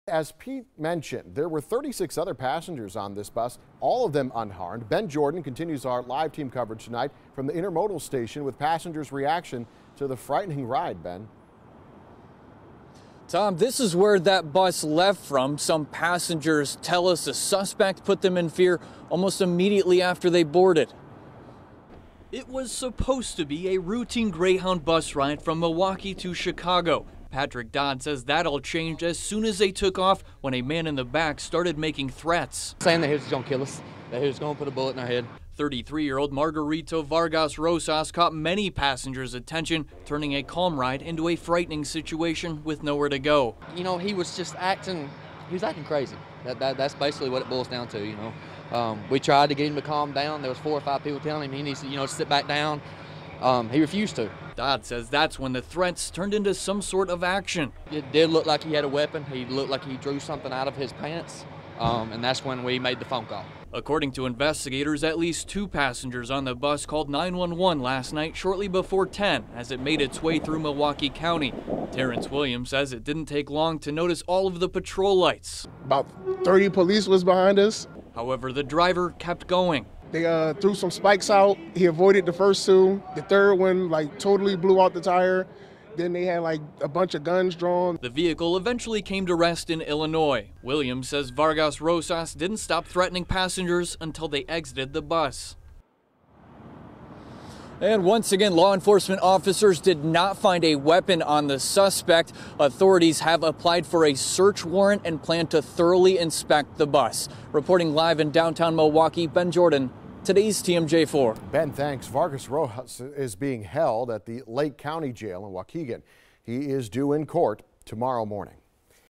Passengers say man on Greyhound bus threatened to kill them
Chilling moments for passengers on a Greyhound bus headed from Milwaukee to Chicago Friday night after a man claimed to have a gun and threatened to kill those on board. Investigators say that suspect was in the United States illegally. He lives in Illnois, but was working at a restaurant in Milwaukee.